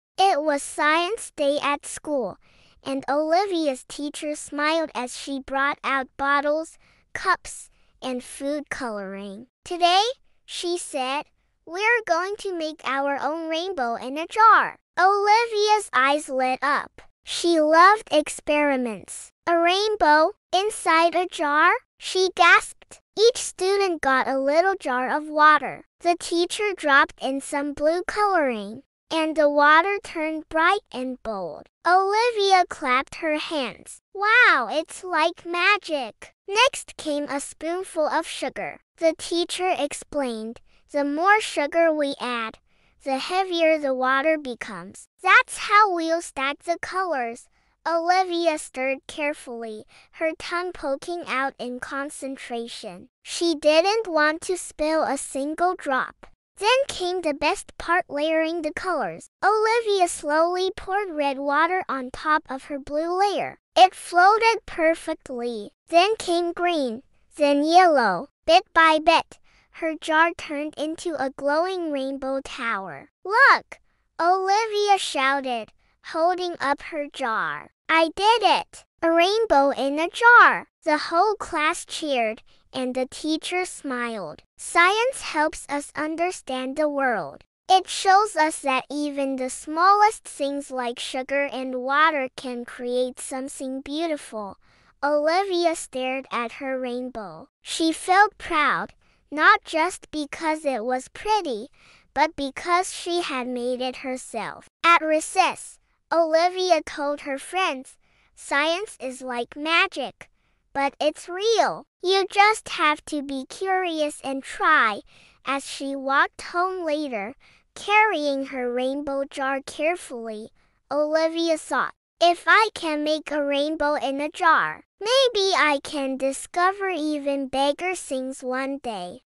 Olivia and the Amazing Science Experiment 🌈 | Kids Story
Join Olivia on Science Day at school as she learns how to make a rainbow in a jar! 🌈✨ With a little sugar, water, and lots of patience, Olivia discovers that science is full of fun, surprises, and magical discoveries. Perfect for children who love stories, learning, and imagination! 💡📚